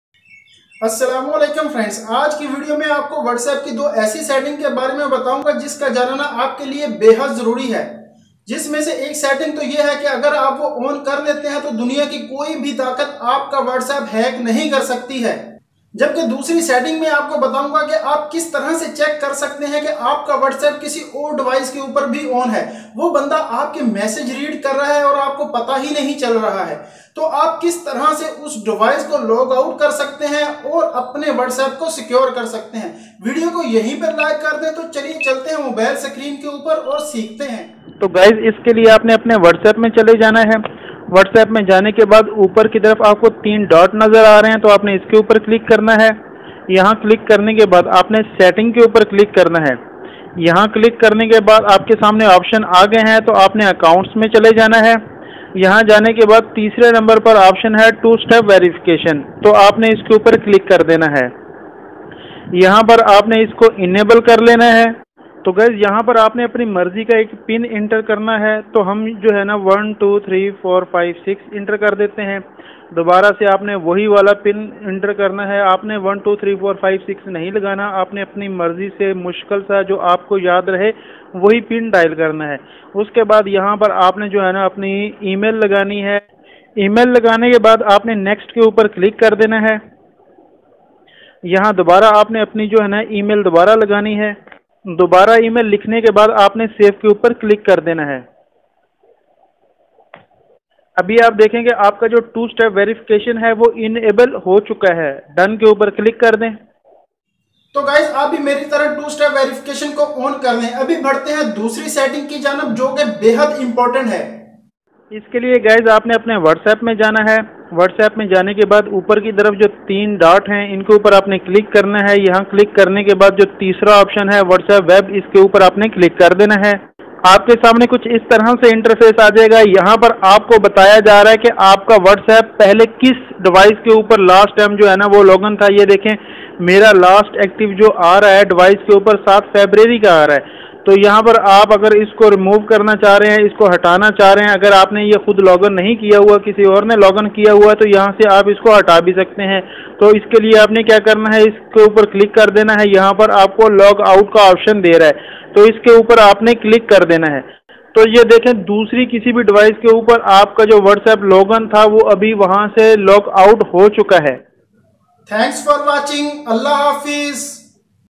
How to Protect your WhatsApp account | Technical Shahid
Friends is video me main apko WhatsApp ki 2 Settings ki information don ga, jis sy ap apny WhatsApp ko hacker's sy bacha sakty han or agr apka WhatsApp kisi device pr login ha or apko pata nahi ha to usko b logout kr sakty han.